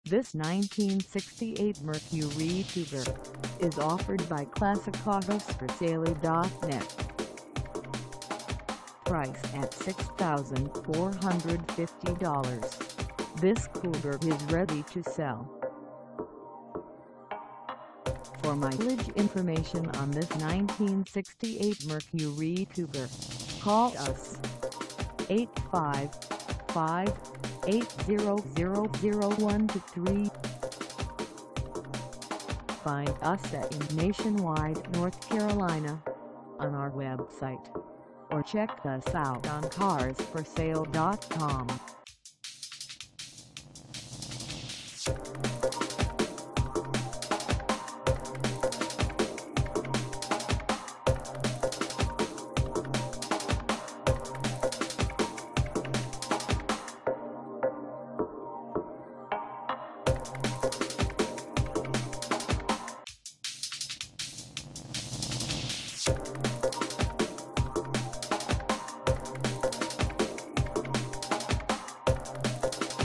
1968 Mercury Cougar for sale in Nationwide, NC 27603 at Cla
This 1968 Mercury Cougar  is for sale in Nationwide, NC 27603 at Classicautosforsale.net.
 392090
Engine: Unspecified
Transmission: Automatic
Color: Unspecified
Mileage: 0
Contact Classicautosforsale.net Nationwide, NC 27603 to test drive this 1968 Mercury Cougar today. List your classic . The best classic car advertising on the planet!